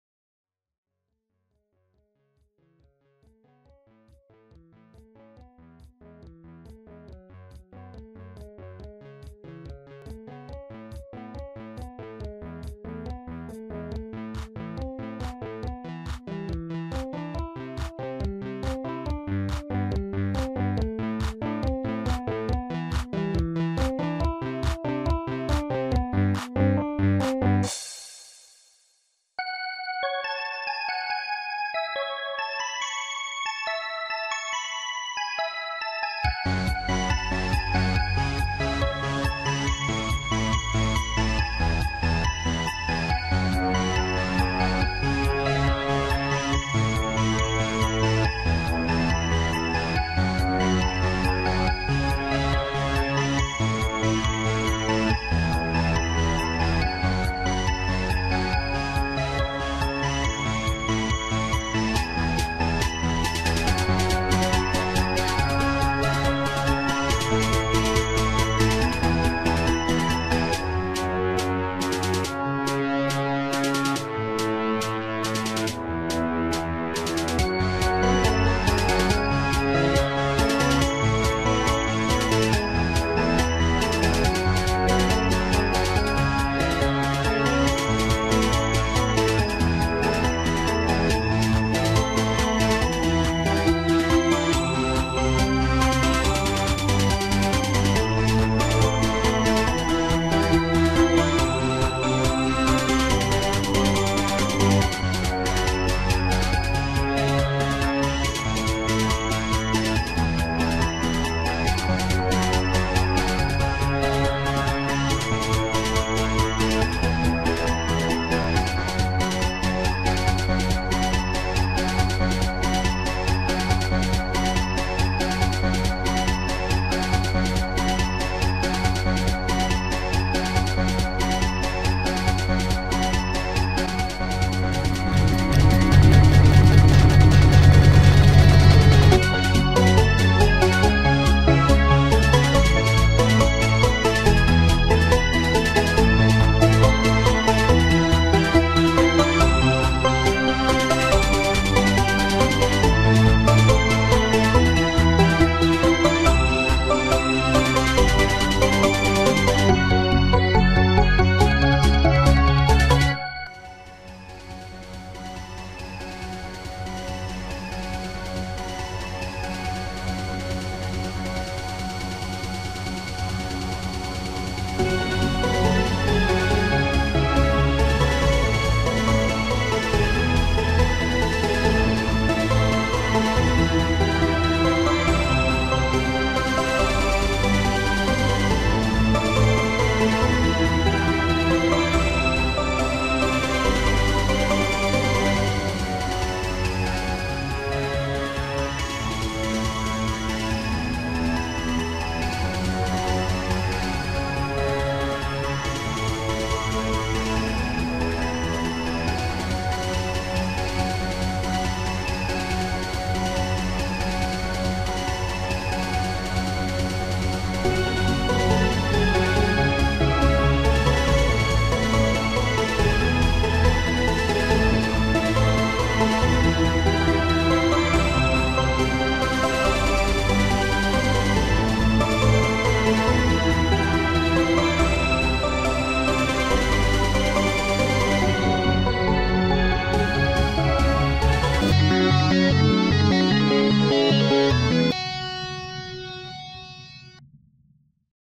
Final Basshunter Remix
In My Opinion, This Is The Basshunter To End All Basshunters I Mashup 3 Different Basshunters Into One Single Remix. And It Sounds Amazing.